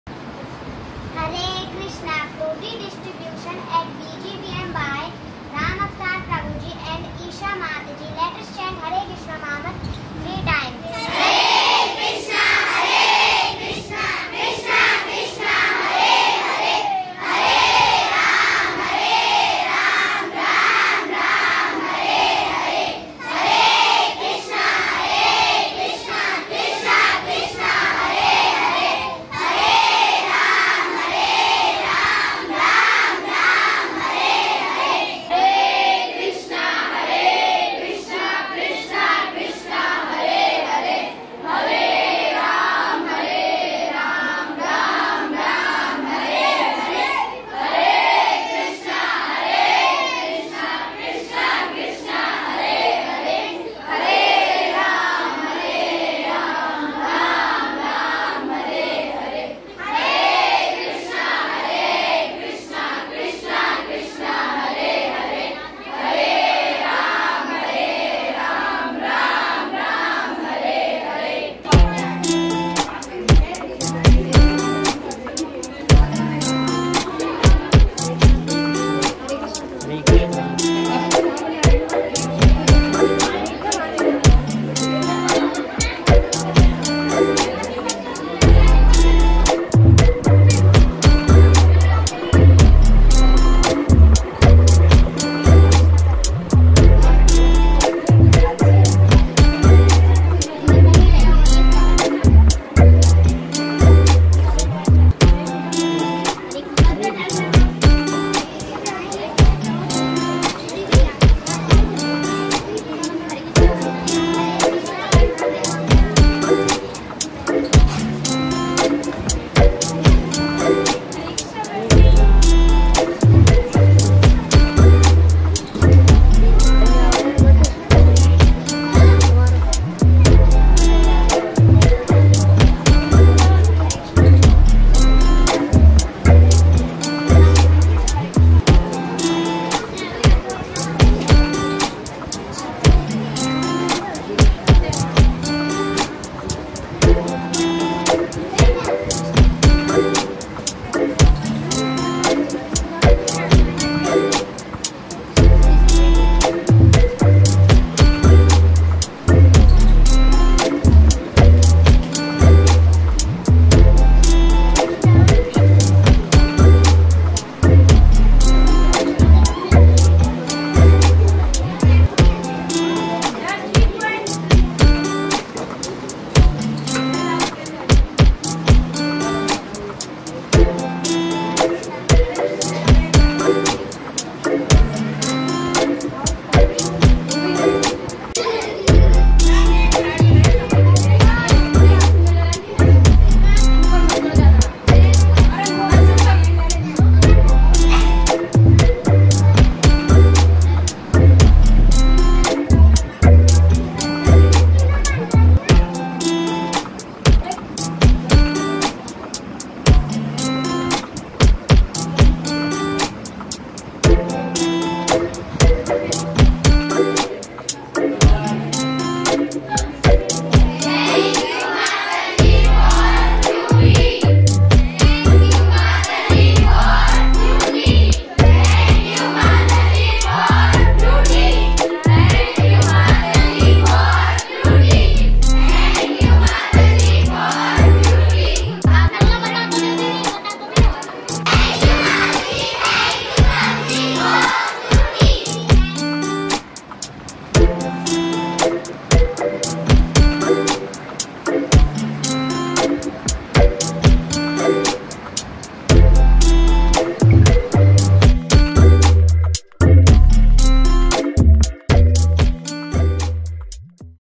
Frooty Distribution | Ram Avtar Prabhuji and Isha Mataji | BGVM Nandgaon | NEC India
Thank you so much to Ram Avtar Prabhuji and Isha Mataji for the Frooty juice distribution in BGVM Nandgaon.